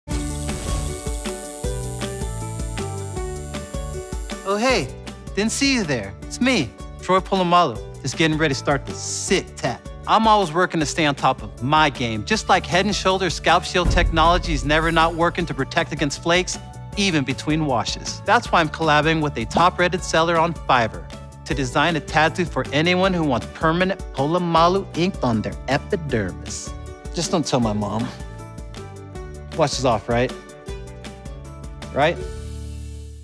Troy Polamalu & Fiverr: Tattoo Gig
Want to get inked by Troy Polamalu?  In collaboration with a top-rated seller on Fiverr.com, Troy is designing your next one-of-a-kind tattoo.

Just like Head & Shoulders Scalp Shield Technology is never not working to give you up to 100% dandruff protection day & night, Troy is always working  to deliver a tattoo design you can't wait to ink.

to apply for the chance to work with Troy and a Fiverr pro on your specialty tattoo project!